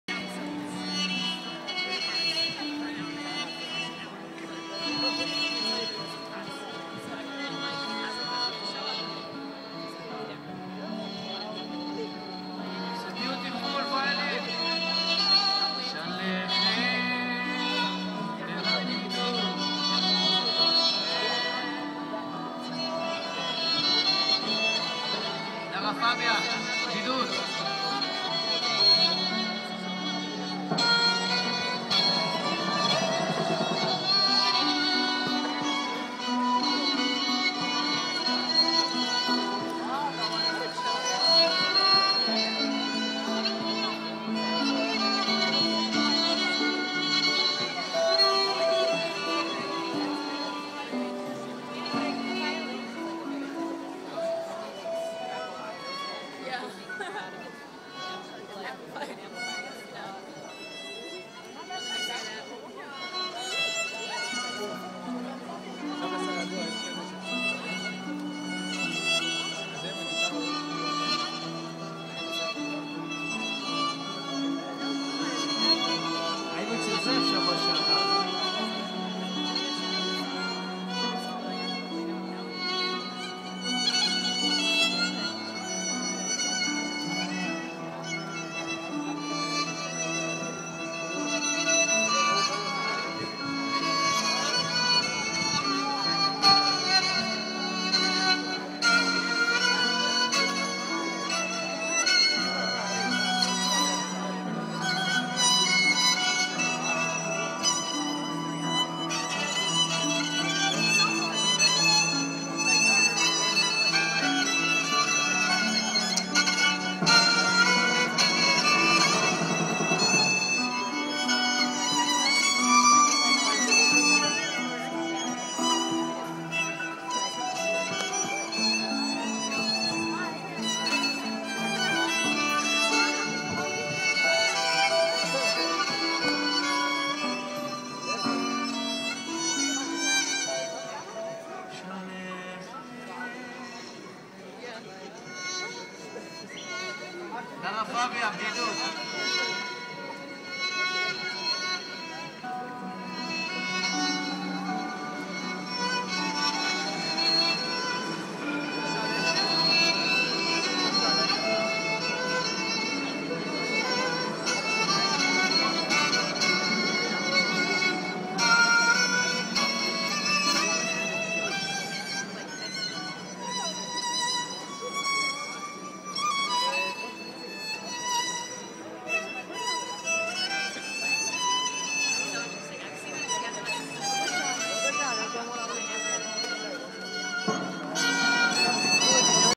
Jerusalem has Talent Ben Yehuda Street
playing music on Ben Yehuda street